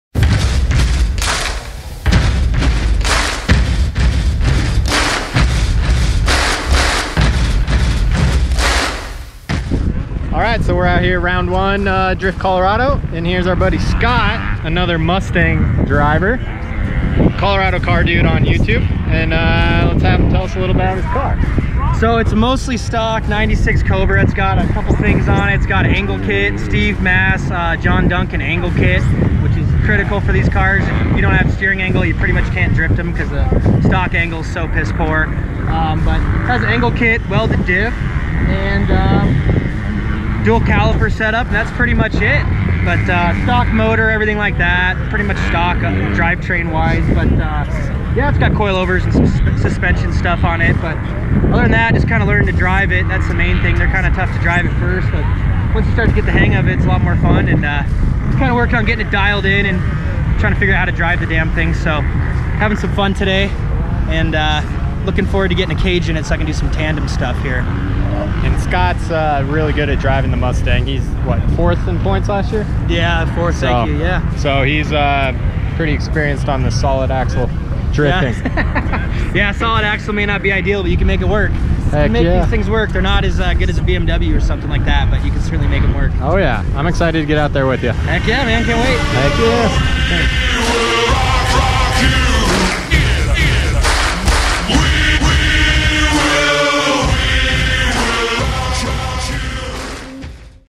Checking our Cocardude's Cobra Mustang Drift Car
Scott shows us around his Cobra Mustang at the drift Colorado round 1 event held at IMI motosports complex in Erie, Colorado. Scott is a super nice dude with a set up grassroots driftstang and he knows how to drive it. Super smooth behind the wheel and a very consistent driver.

Social Media -